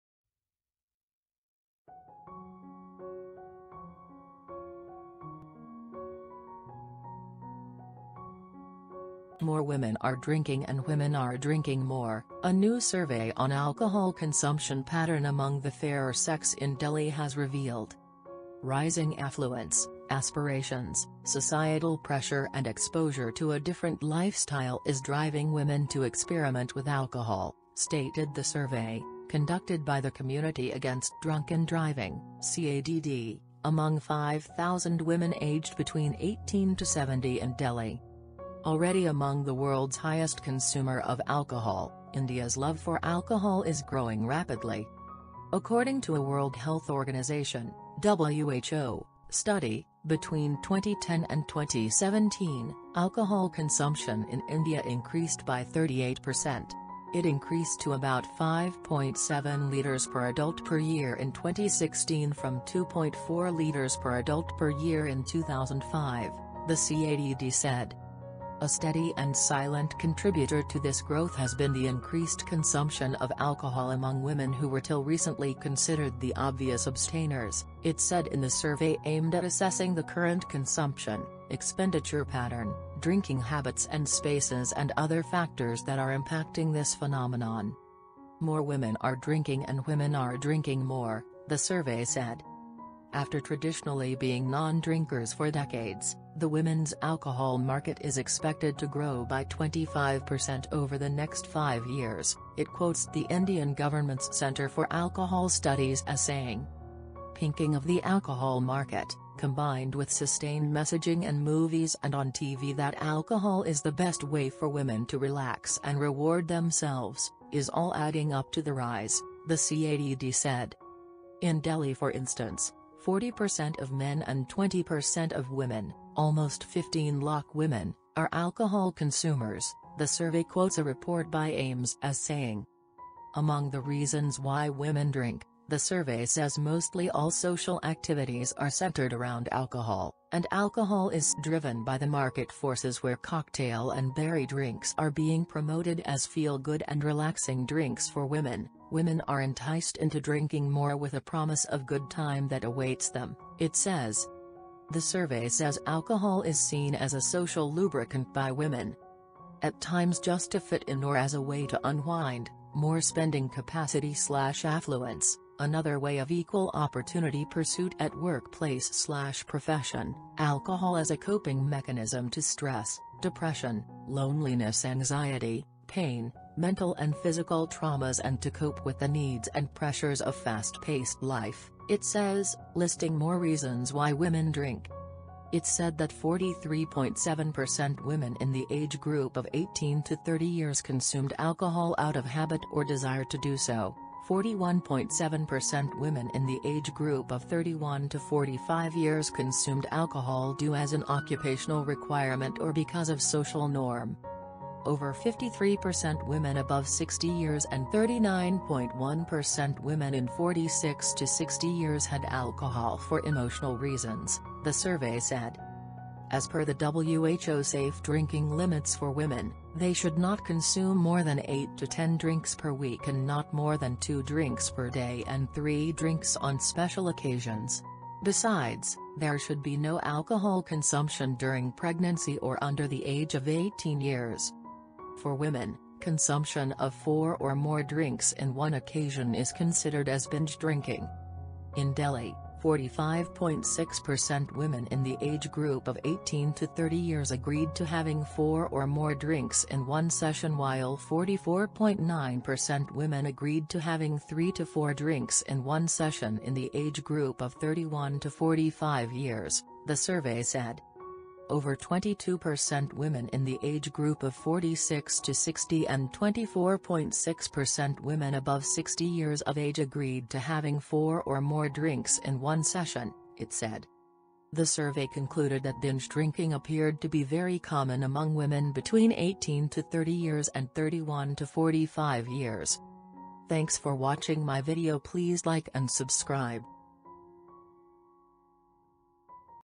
Why indian Women's increased alcohol consumption ?
"More women are drinking and women are drinking more", a new survey on alcohol consumption pattern among the fairer sex in Delhi has revealed.
Rising affluence, aspirations, societal pressure and exposure to a different lifestyle is driving women to experiment with alcohol, stated the survey, conducted by the Community Against Drunken Driving (CADD) among 5,000 women aged between 18 to 70 in Delhi.